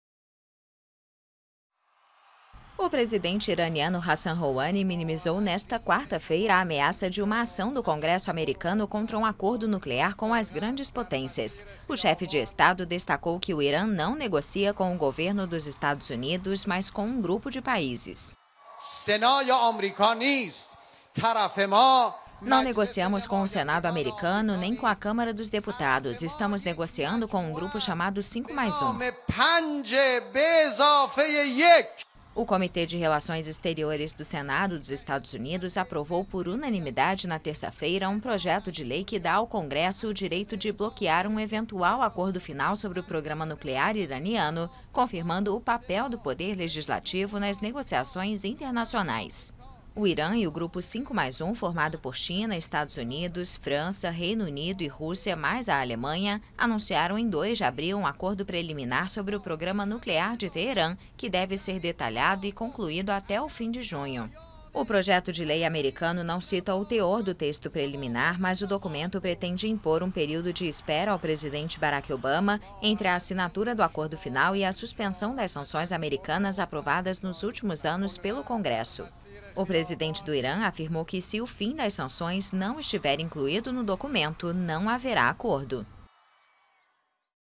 Irã diz não negociar questão nuclear com Congresso dos EUA
O presidente iraniano Hassan Rouhani afirmou nesta quarta-feira que seu país não negocia a questão nuclear com o Congresso dos Estados Unidos e sim com as potências do grupo 5+1.